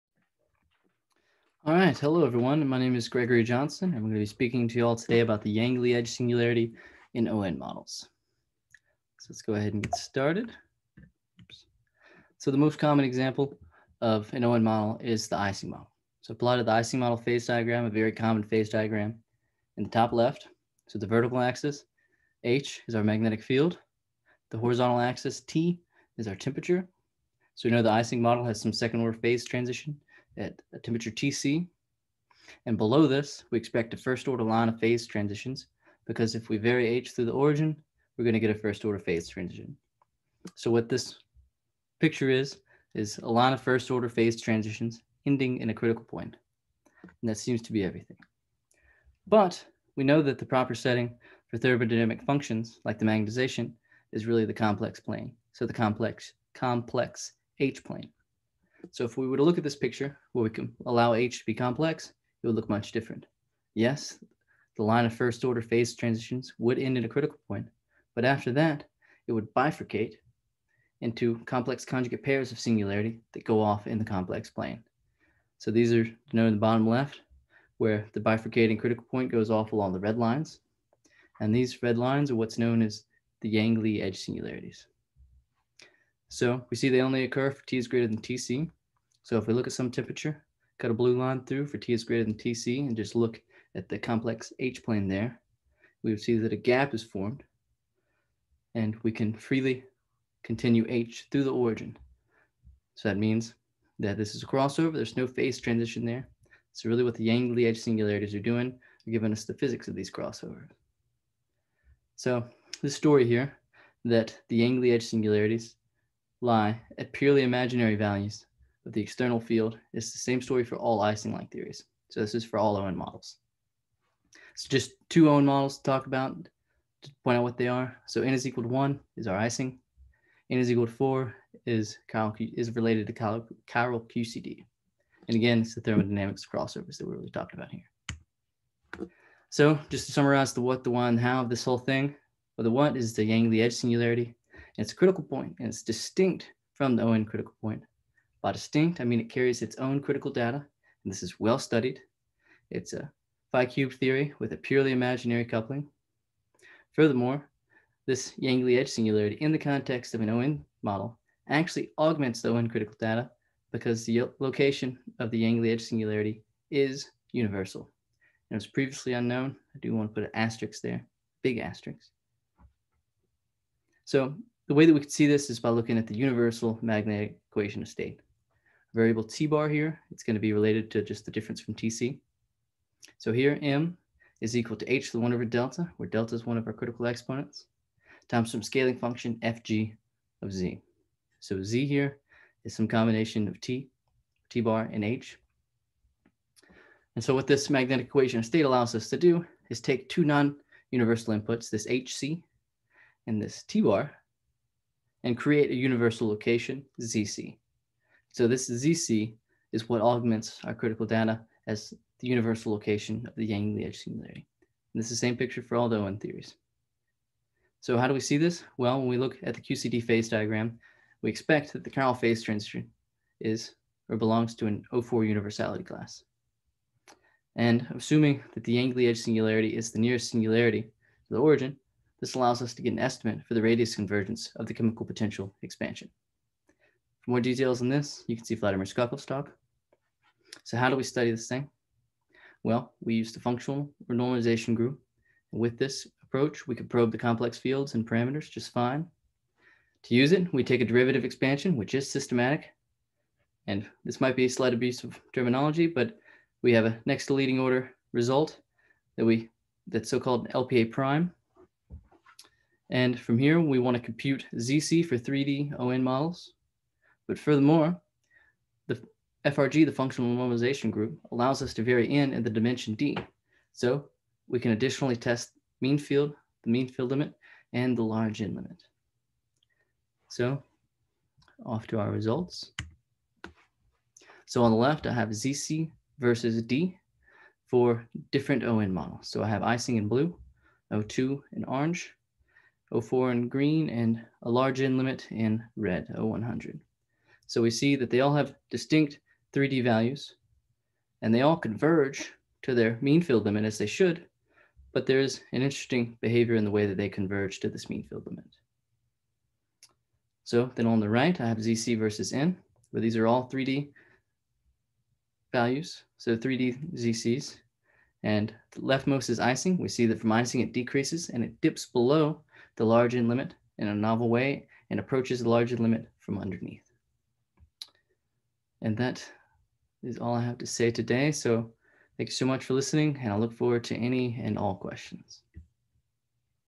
The Universality of Yang-Lee Edge Singularities in O(N) Models, Gregory Johnson, NCSU
As a system is tuned away from a second order phase transition into the crossover region, the critical point splits into a pair of complex conjugate singularities lying at complex values of the system parameters - the Yang-Lee edge singularities. These singularities can be treated as ordinary
critical points belonging to a specific universal class. It has only recently been realized that these points augment the parent universality class since their locations are universal when written in terms of the relevant scaling variables. For QCD in the O(4) crossover regime, this translates into
a critical point at complex values of the baryon chemical potential. In this talk, we report on the first study of these universal locations for the 3d O(N) universality classes using the functional renormalization group. We additionally vary N and the dimension to obtain the analytic results of mean field and the large N limit.